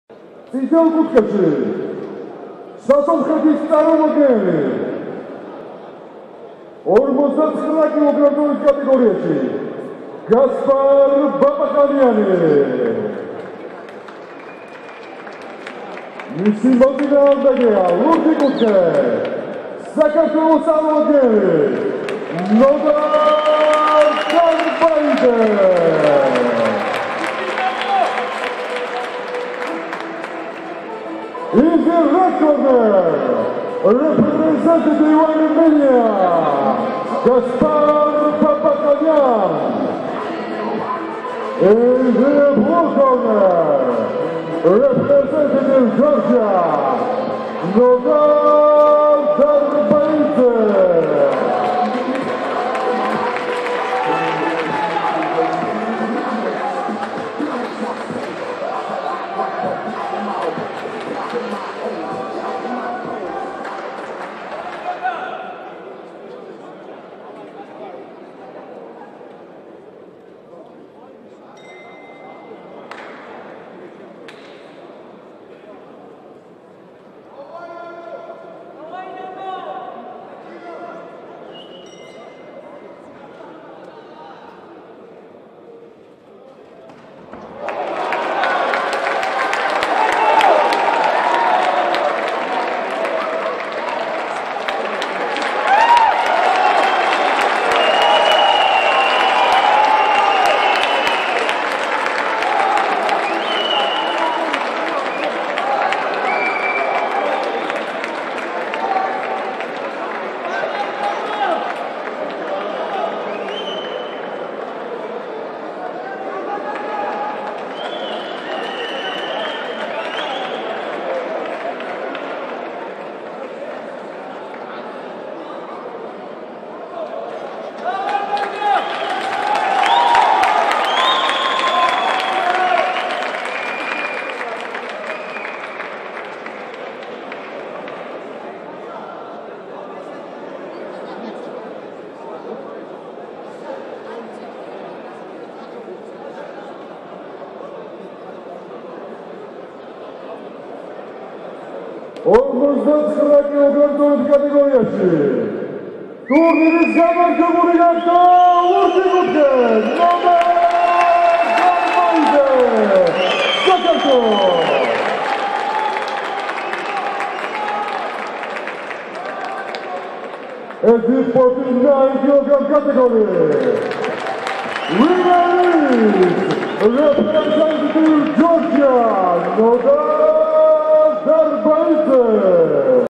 BOXING FINAL14 april 2017(49kg)RED Babaxanian Gaspar ARM VS BLUE Darbaidze Nodar GEO .
სერგო დავლიანიძის რუბრიკა - კრივი და ქართველი მოკრივეები.
БОКС- ФИНАЛ-14 апреля 2017 г. (49кг) Бабаханян Гаспар  -Армения  VS  Дарбаидзе Нодари - Грузия. Тбилиси - Грузия. Международный турнир по боксу имени ЗМС СССР ДАВИДА КВАЧАДЗЕ,чемпиона Европы и трёхкратного чемпиона СССР в полутяжёлом весе.
ლეგენდარული სპორტსმენის, კრივში ევროპის პირველი ქართველი ჩემპიონისა და საბჭოთა კავშირის ჩემპიონატის სამგზის გამარჯვებულის დავით კვაჭაძის სახელობის საერთაშორისო ტურნირის - ფინალური ორთაბრძოლა 49 კგ-მდე წონით კატეგორიაში. გასპარ  ბაბახანიან VS  ნოდარ დარბაიძე.